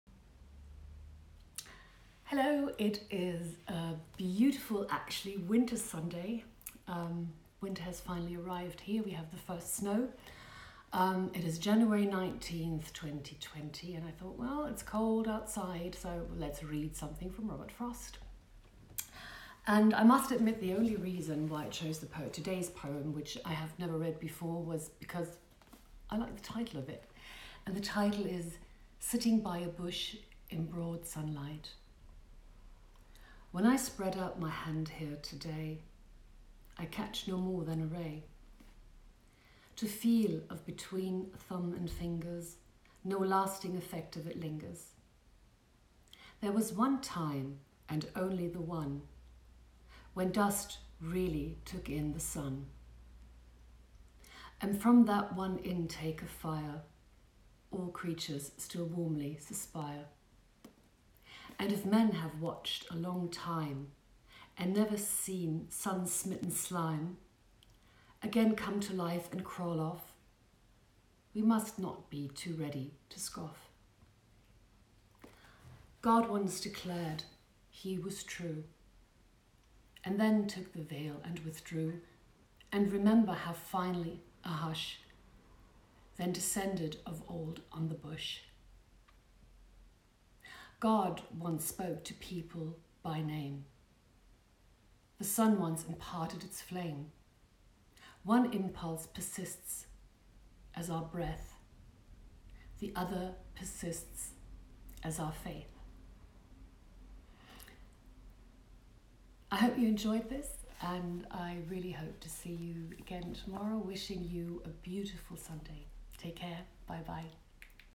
Poetry Challenge #17
As we have the first snow here I decided to read Robert Frost and I chose the Poem for its title:
Sitting In A Bush In Broad Sunlight